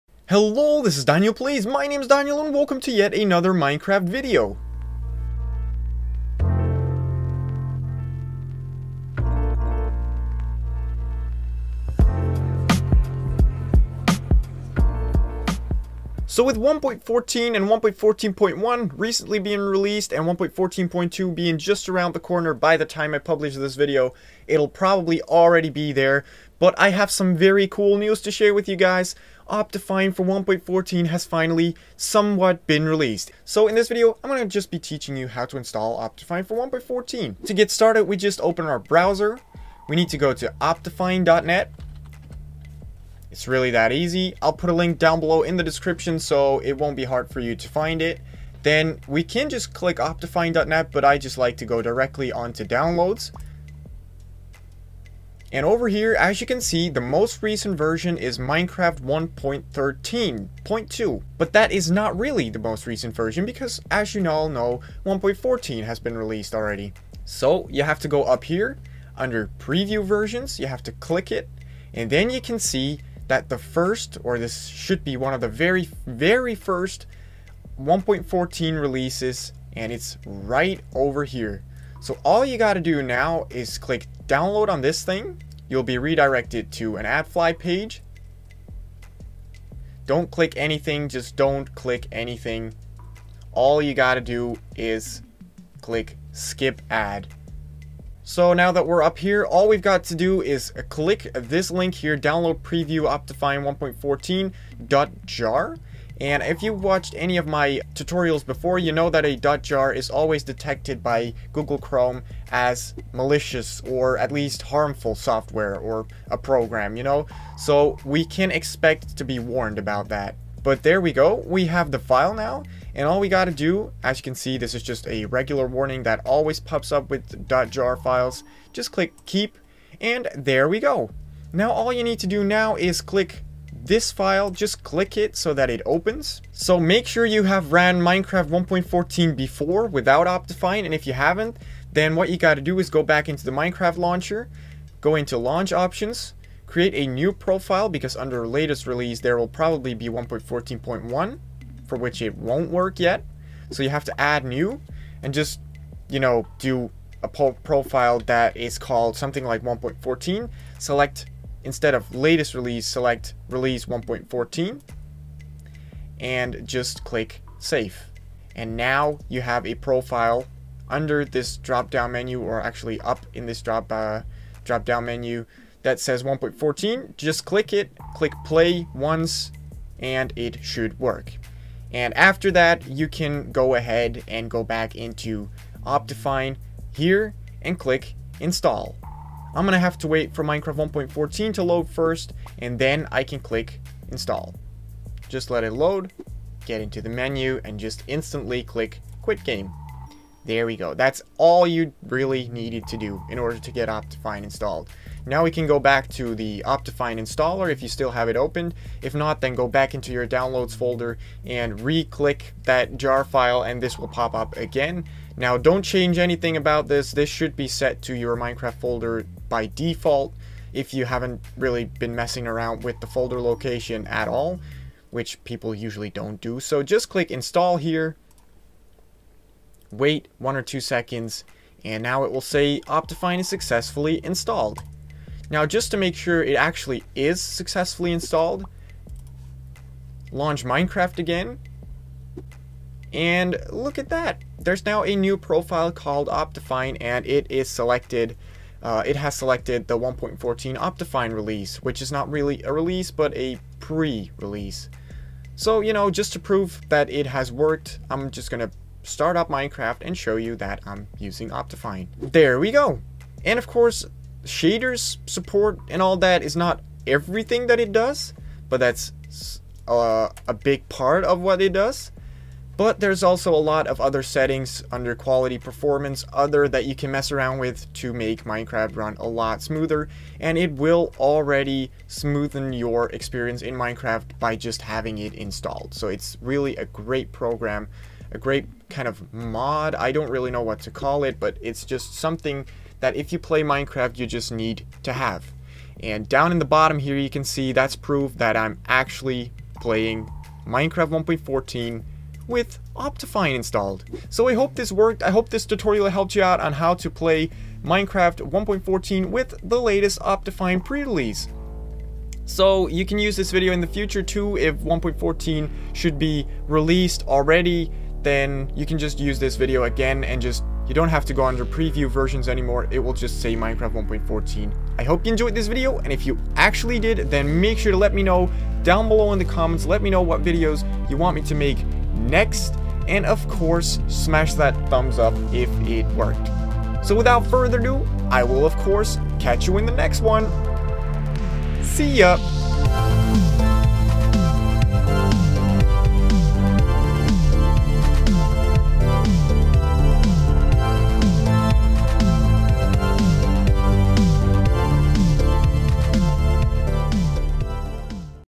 How To Install Optifine for Minecraft 1.14.2 | Minecraft Tutorial
Hello and welcome to yet another tutorial! In this video I'll show you how to install Optifine for Minecraft 1.14.2. Optifine for 1.14.2 has only just been released and could therefore still be in testing phase. Optifine is an amazing mod for Minecraft that gets rid of lag spikes, increases FPS and allows you to customise lots more. But it can be tricky to install it, so if you're running in to problems, during or after watching this video, be sure to leave a comment and I and other people will probably be able to help you out!